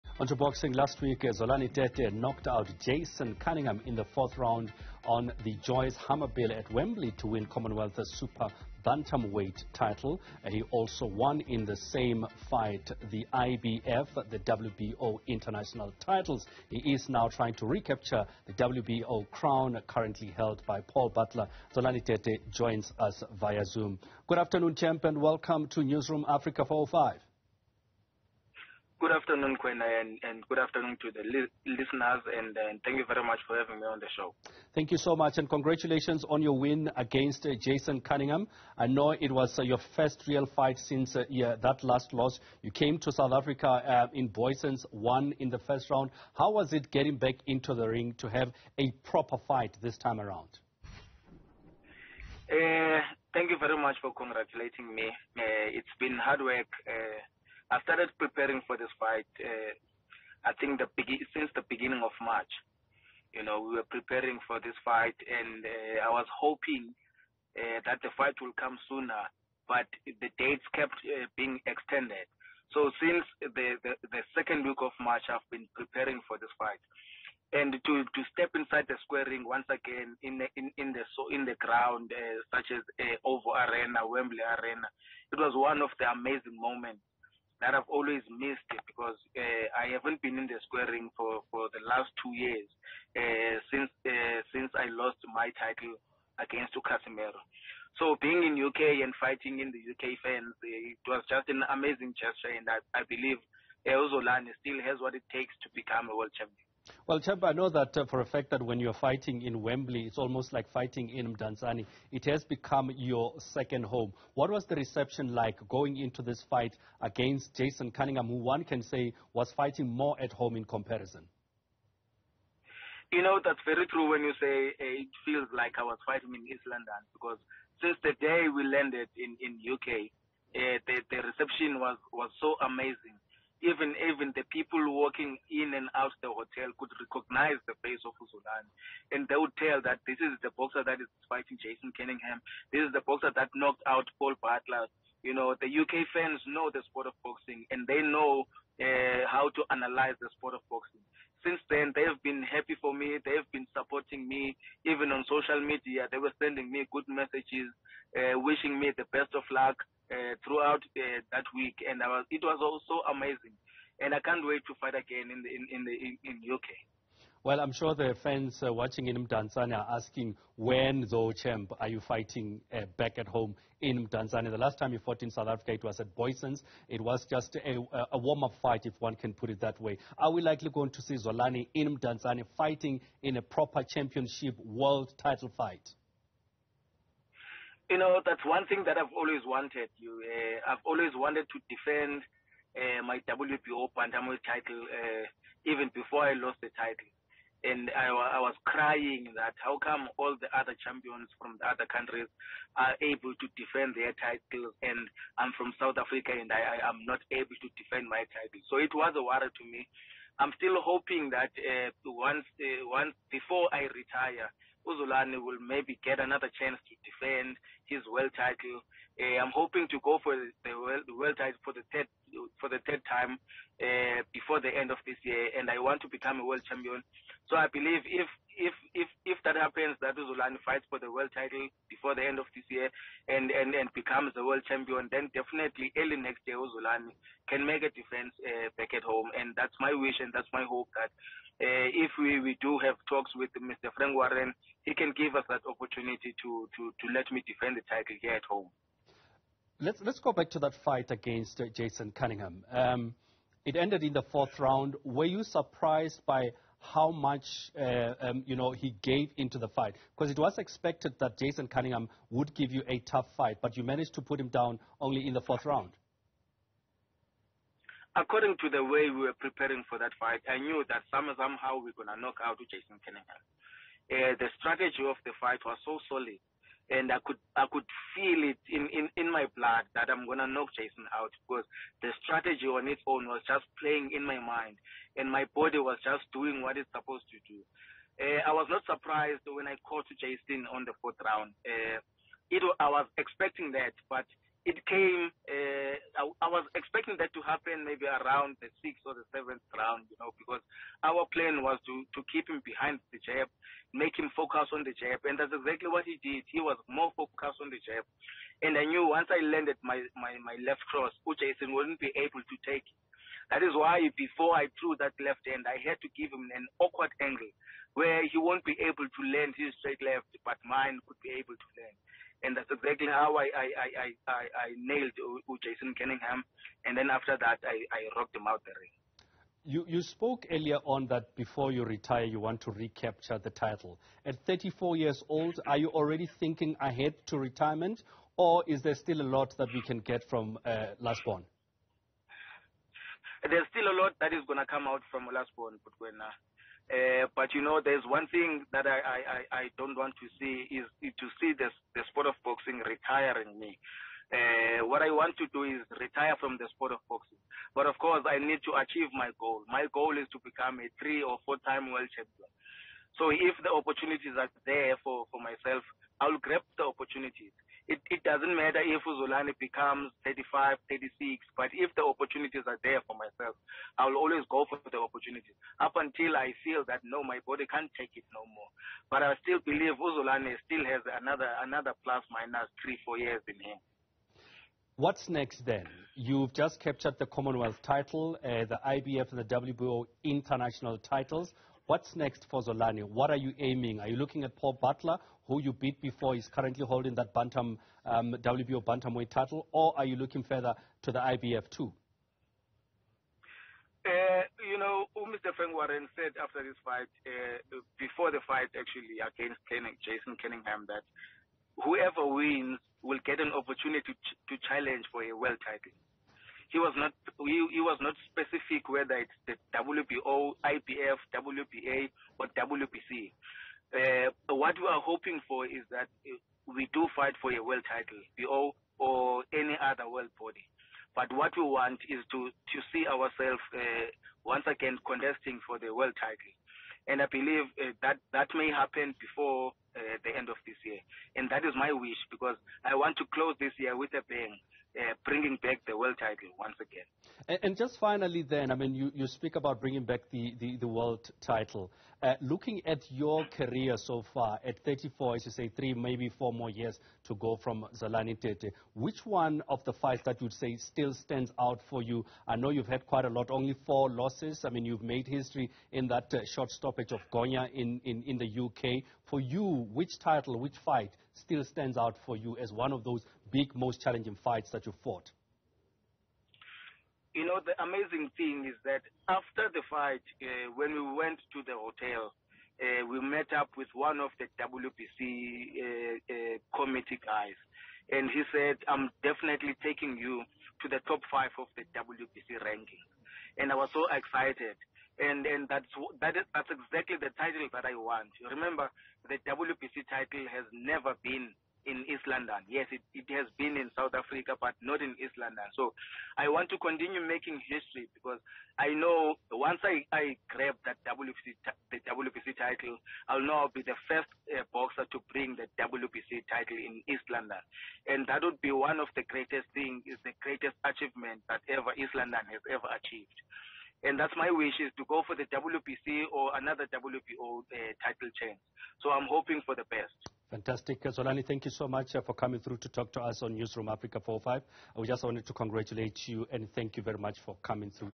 Boxing champion Zolani Tete recaptures the Commomwealth Super Bantamweight match
Last week, Zolani Tete knocked out Jason Cunningham in the fourth round on the Joyce-Hammer bill at Wembley to win the Commomwealth Super Bantamweight title. Tete is a former IBF and WBO Bantamweight champion and he recapture the WBO crown currently held by Paul Butler.